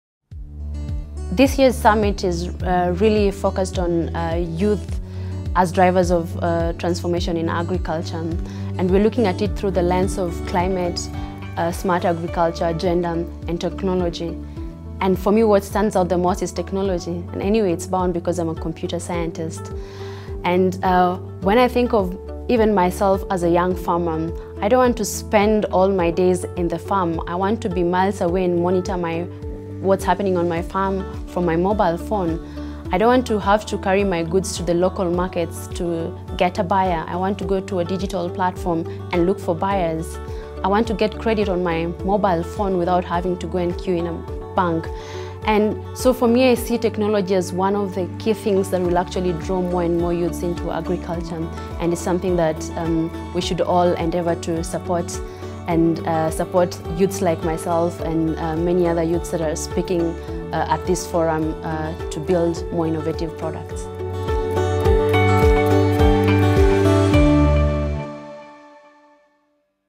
Youth driving agricultural transformation in Africa: Rita Kimani (part 1)
In this video (part 1), Rita Kimani, Co-founder and CEO of FarmDrive, highlights the advantages of applying technology into agricultural production.

Rita is a youth delegate at the 2017 MasterCard Foundation Young Africa Works Summit in Kigali, Rwanda.

Rita believes that youth can drive transformation in agriculture. She introduces themes that contribute to agricultural transformation, including gender, technology and climate-smart agriculture.

Rita believes that technology is one of the key themes that will draw more youth to agriculture, and it is something that we should all endeavor to support.